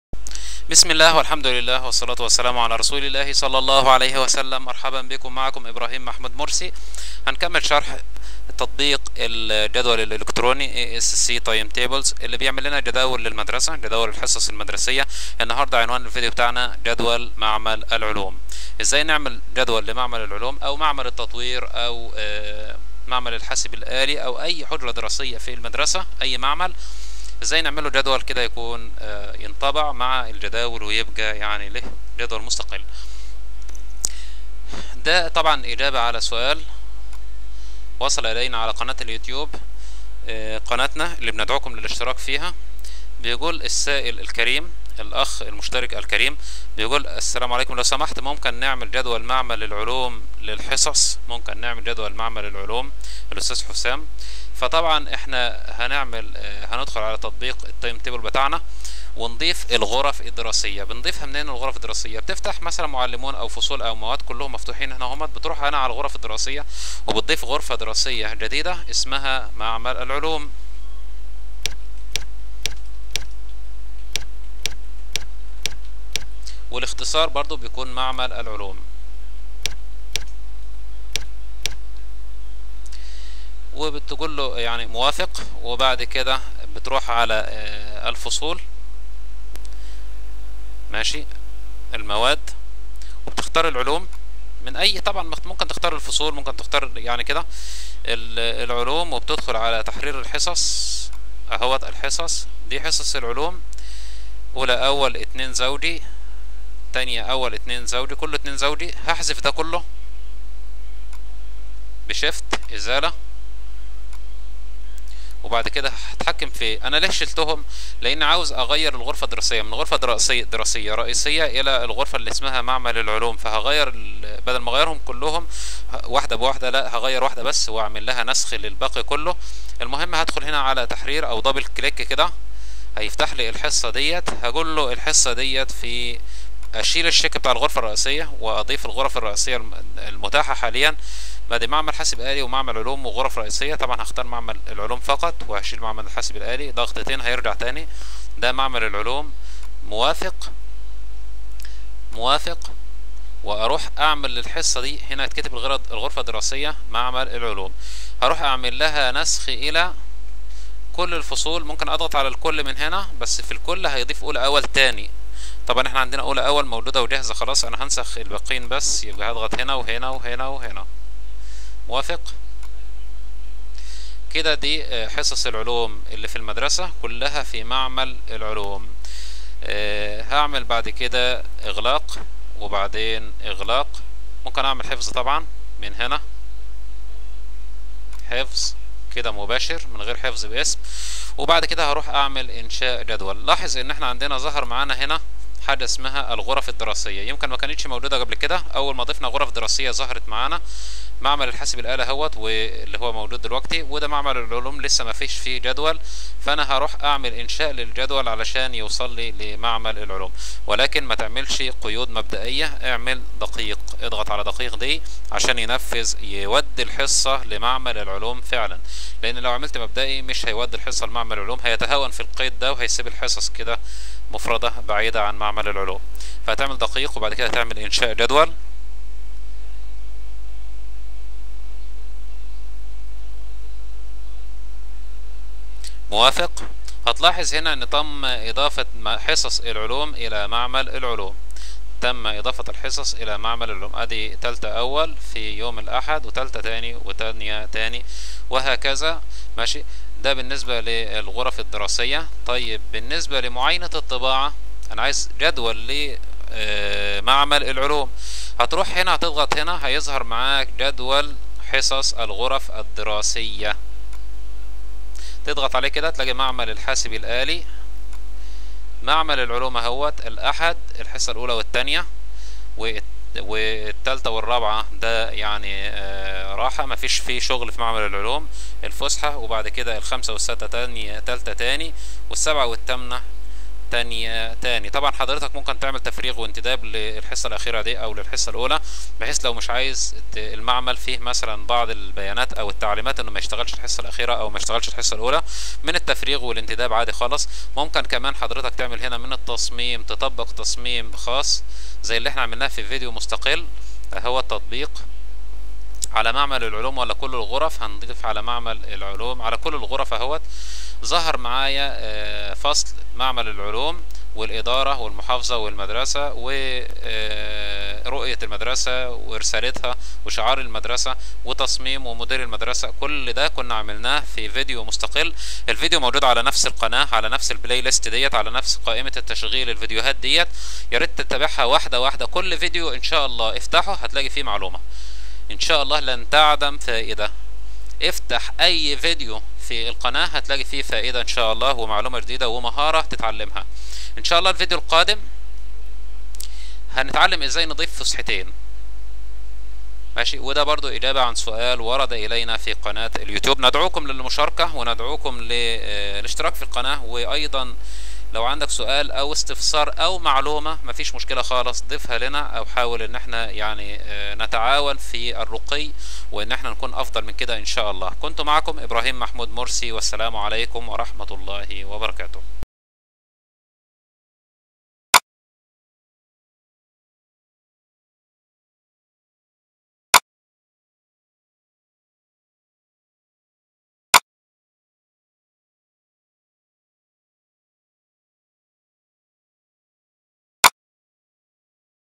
29 الجدول المدرسي aSc timetables جدول معمل العلوم
جدول معمل العلوم
جدول معمل الحاسب الالي
جدول معمل التطوير التكنولوجي
وغيرها من المعامل والحجرات الدراسية المختلفة
كيف يمكن عمل الجداول السابقة في تطبيق التايم تيبل
هو ما نحاول الاجابة عنه في الفيديو الحالي استجابة لطلب الاعضاء الكرام في التعليقات على الفيديوهات
ندعوكم للاشتراك في القناة والتفاعل والمناقشة الايجابية
نرجو الخير والرشاد
ونسأله سبحانه التوفيق والسداد .